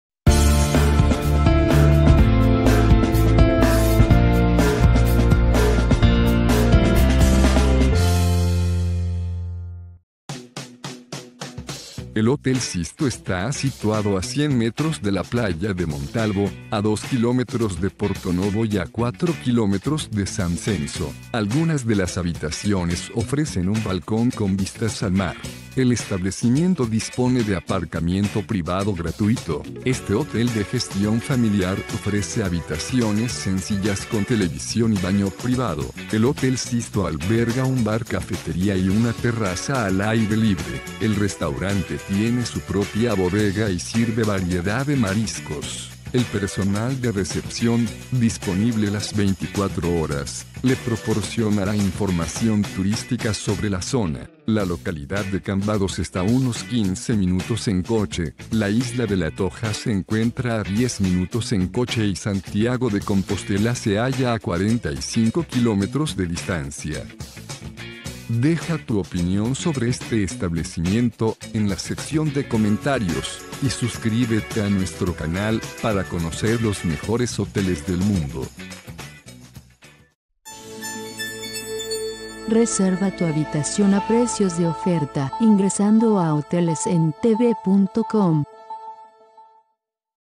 Hotel Sixto en Montalvo
Hotel Sixto de categoría 1 estrellas cuenta con restaurante, bar y recepción 24 horas.